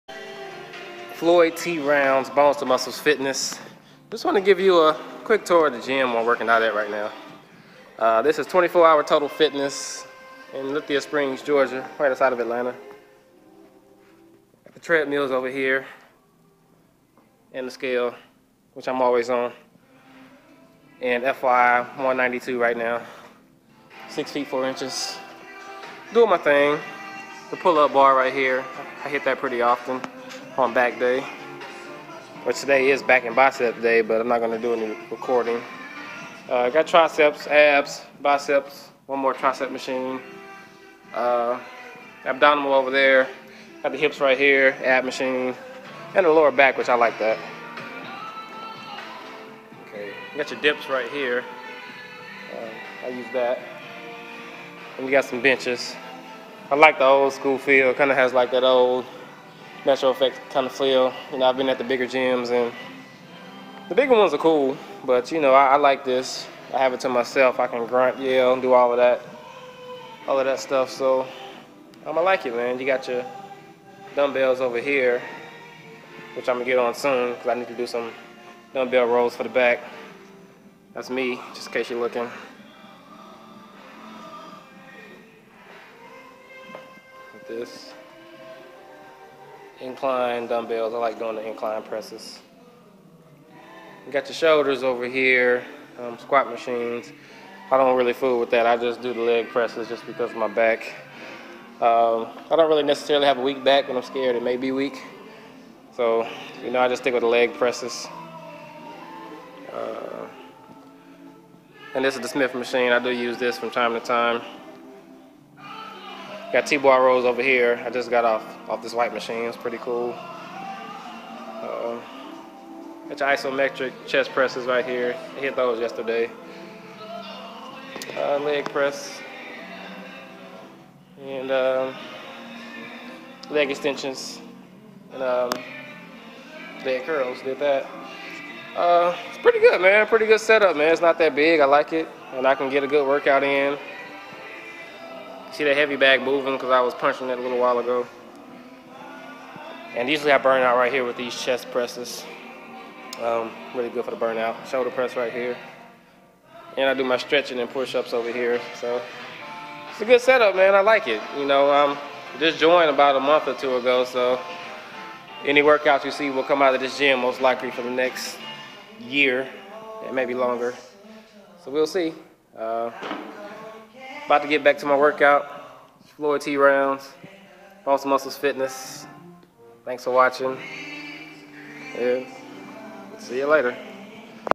My New Gym - 24 Hour Total Fitness, Lithia Springs (Atlanta), Georgia
Just a quick tour of my new gym. Old school but fun.